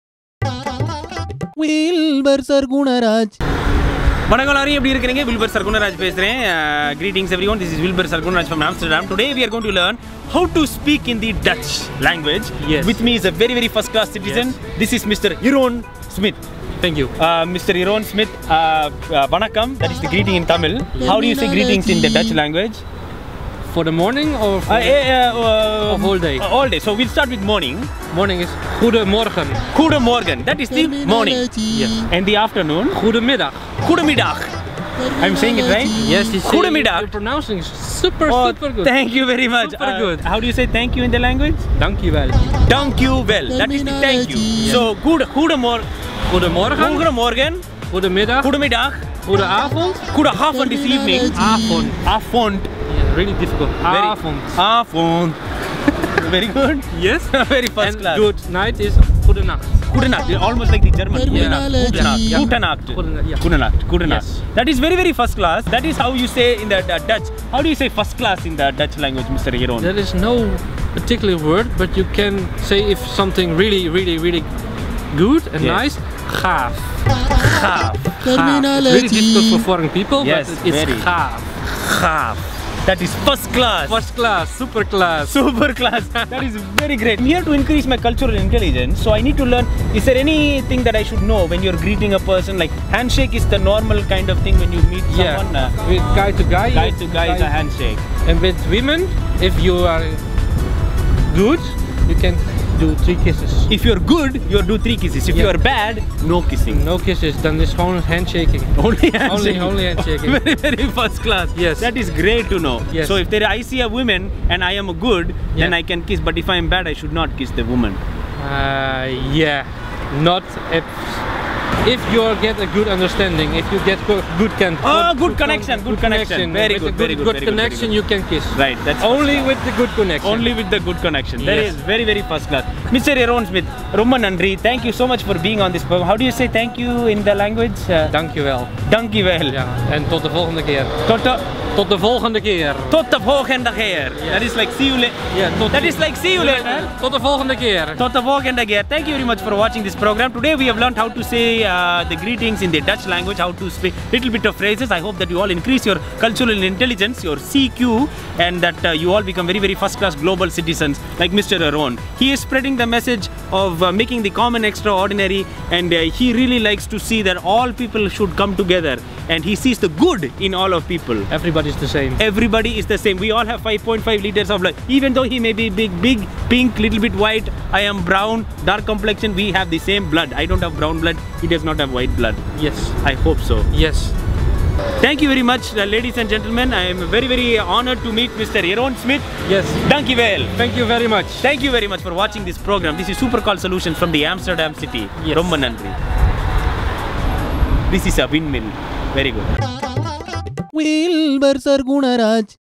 How to speak Dutch-Wilbur Sargunaraj in Holland
Welcome to Super Call Solutions!  In this episode join Wilbur and host Mr Jeroen Smit in Amsterdam as they teach you basic phrases in the dutch language.